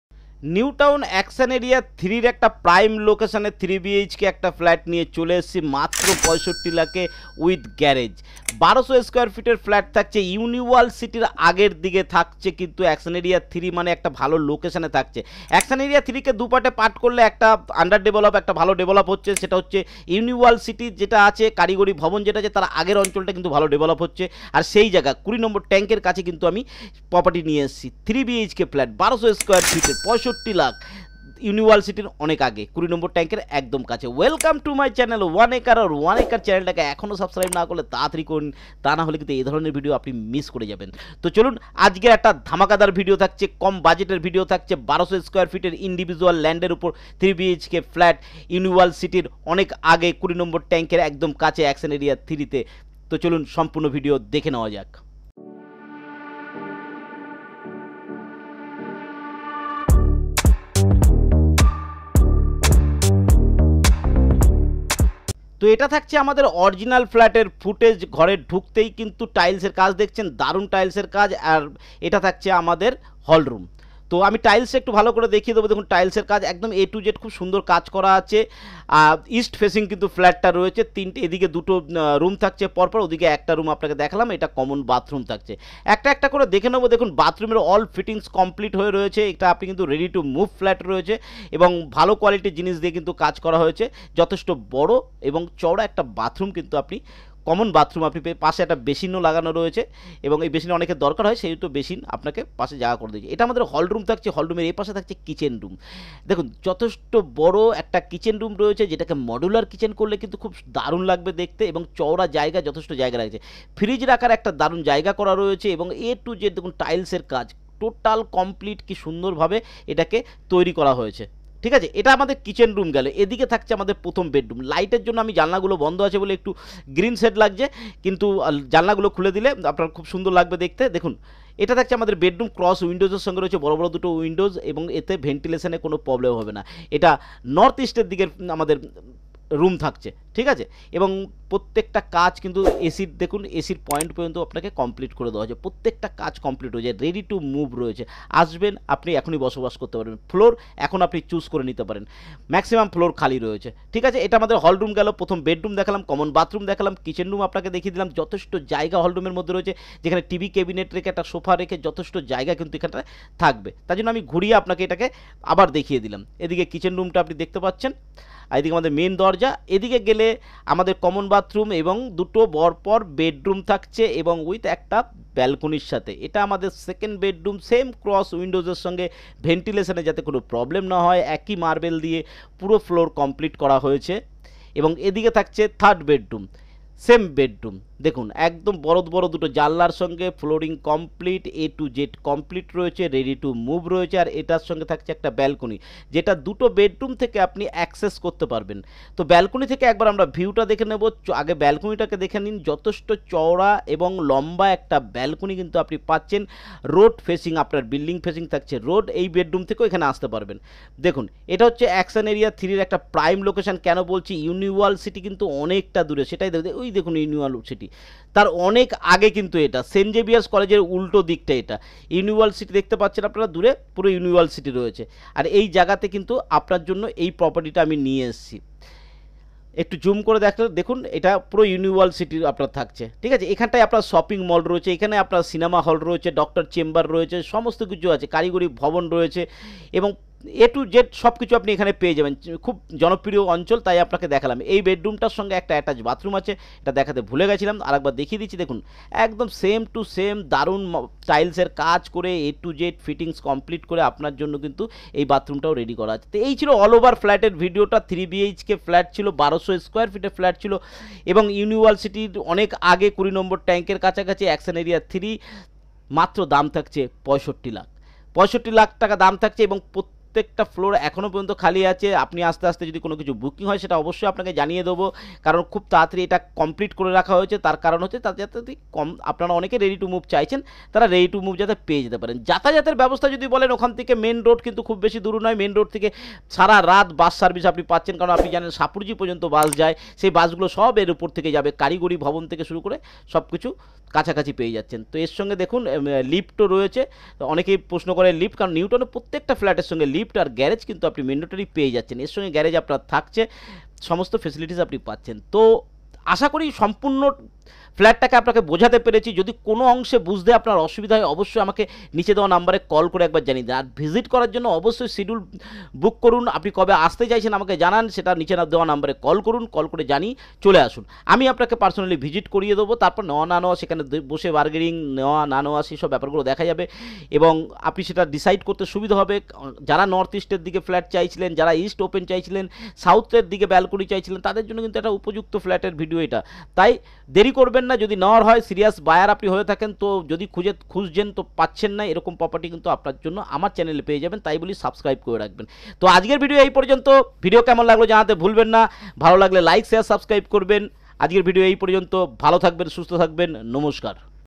3BHK 1200sqft Flat for Sale at Newtown action area-3B | Ready to Move Flat at Newtown
3BHK 1200sqft Flat for Sale at Newtown action area-3B |Ready to Move Flat at Newtown | Near-Uniworld City
✅ Individual Flats, G+4
✅ Location - Newtown action area-3B.
✅ Areas-Super built up -1200 Sqf.
✅ Areas-Carpet -1000 Sqf.
✅ Floor- 2nd and 3rd.
✅Stage- Ready to Move .
✅ Rs-65 lakh
✅ Covered parking
✅ Lift - Yes

👉আপনার ফ্লাট,বাড়ি ,জমি বিক্রি করতে চাইলে আমাদের সঙ্গে যোগাযোগ করুন।
👉হোয়াটস্যাপ করুন এই নাম্বারে - - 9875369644